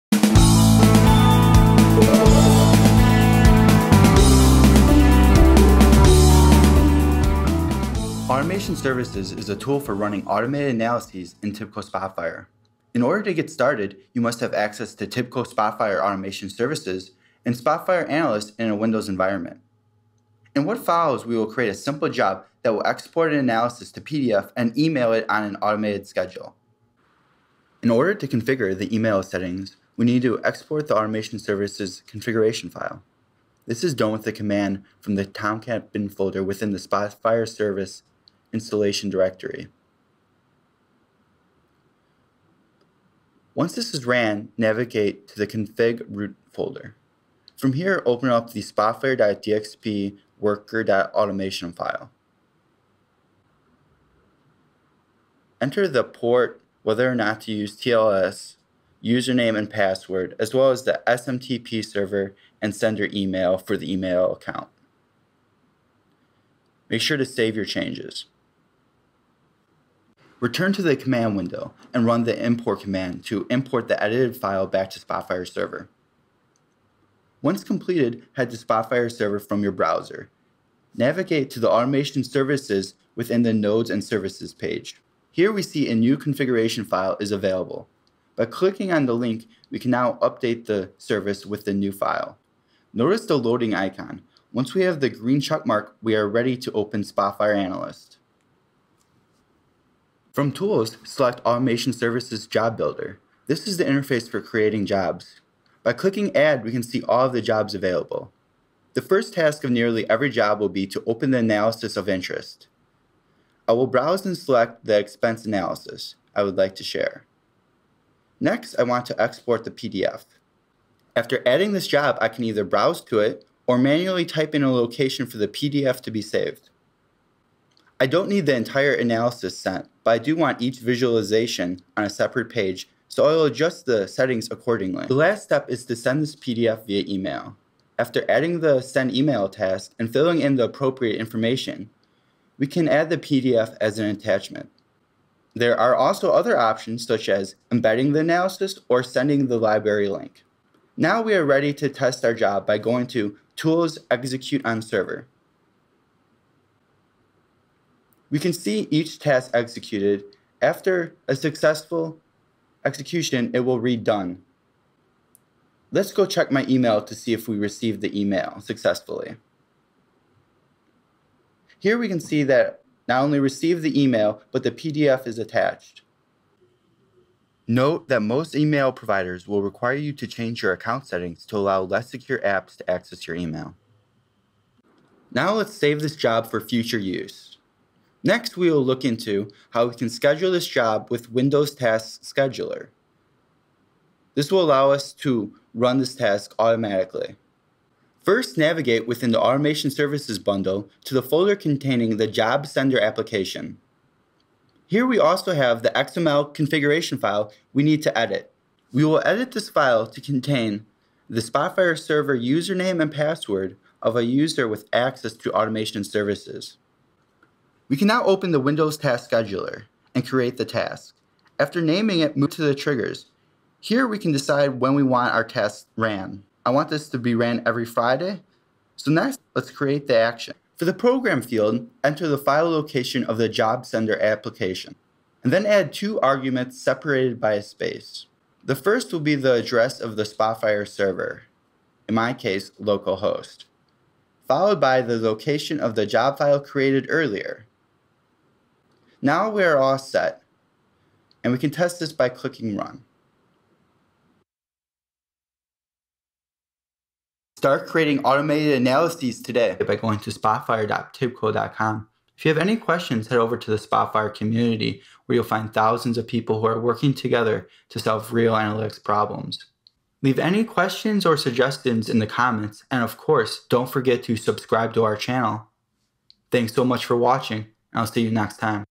How to Create and Schedule an Automation Services Job in Spotfire
Automation Services is a tool for running automated analyses in Spotfire. It lets you automate the creation of analysis files and perform a series of actions. In this video we will create an automation services job that will open an analysis, export to PDF, and email out on an automated schedule.